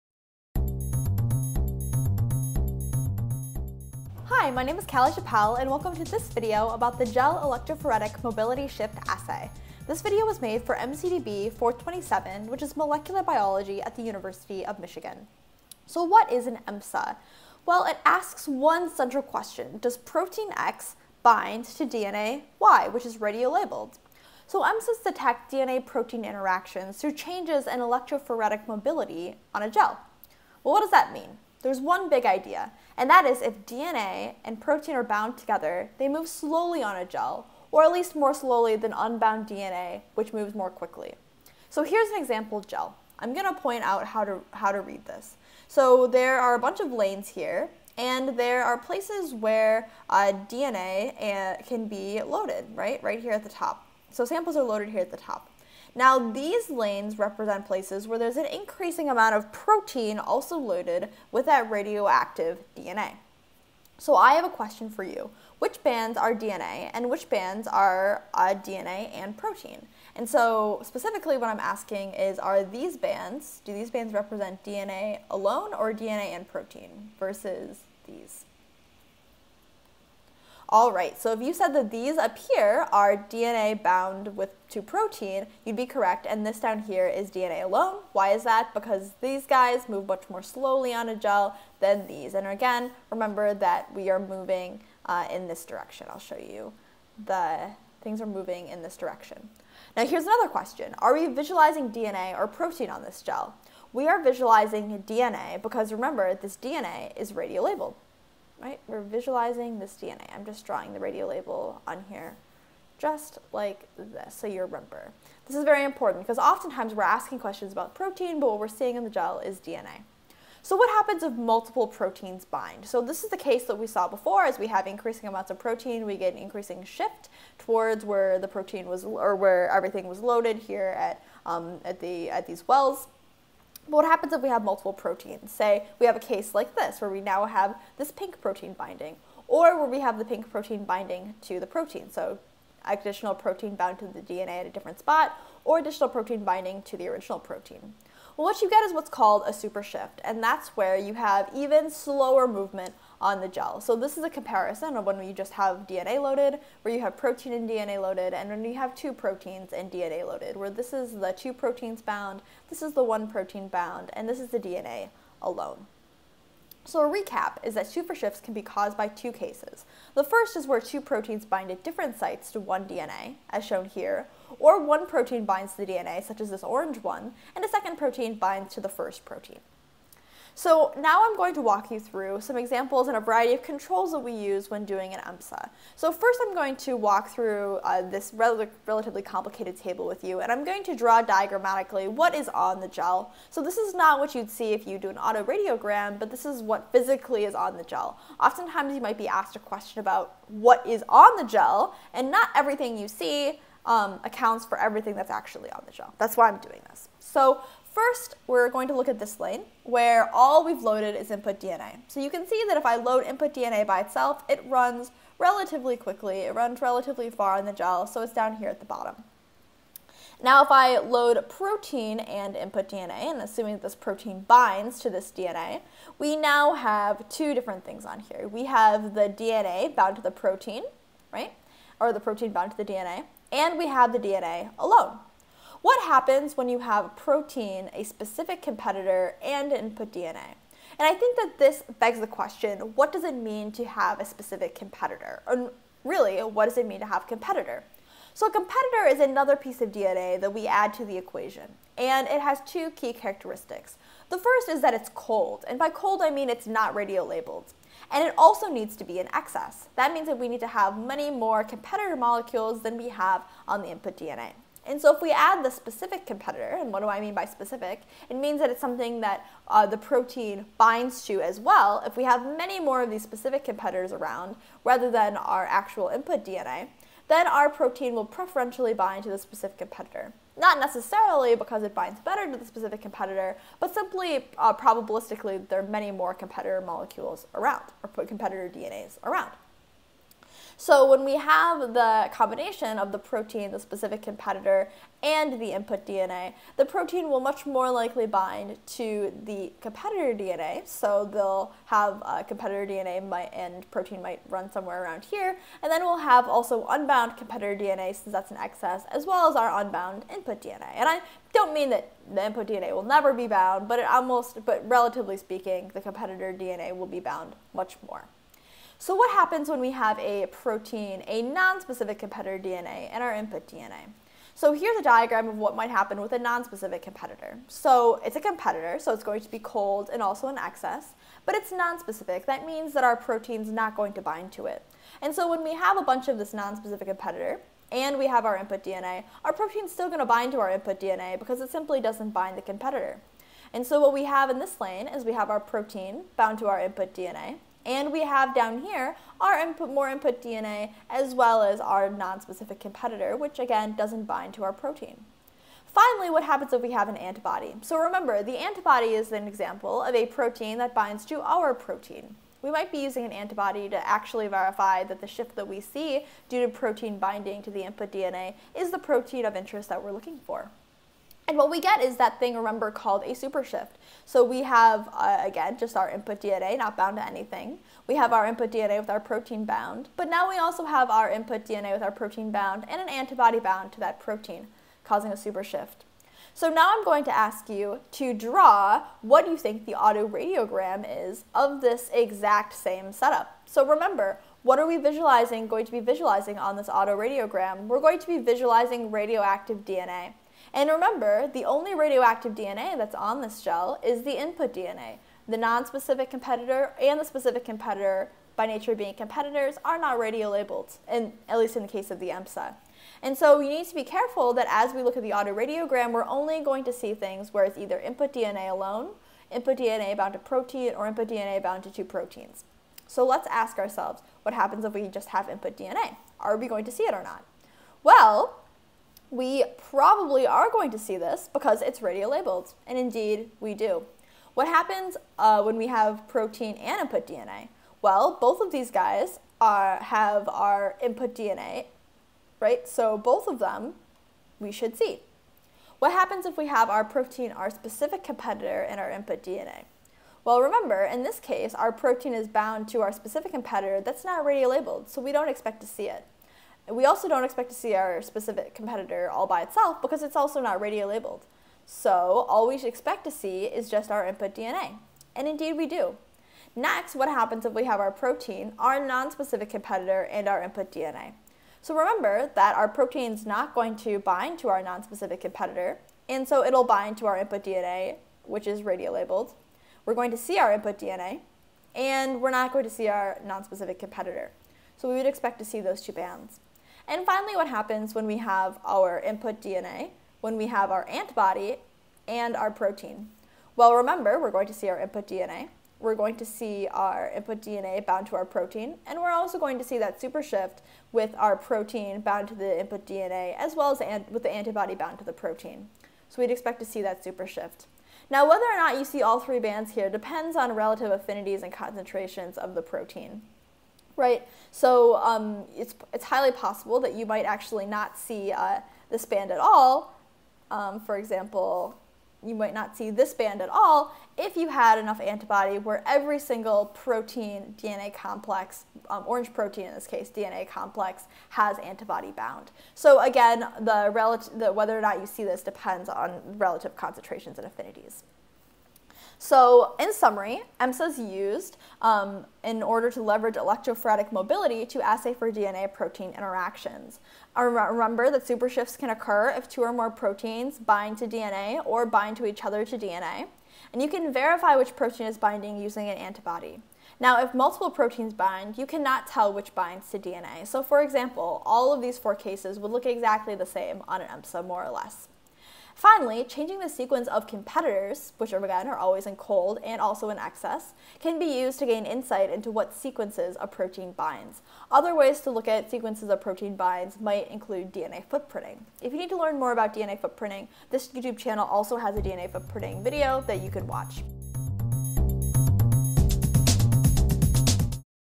EMSA (Electrophoretic Mobility Shift Assay) Fig 5.36
This video is about the Electrophorectic Mobility Shift Assay (EMSA) and was made for MCDB 427 (Molecular Biology) at the University of Michigan.  Figure 5.36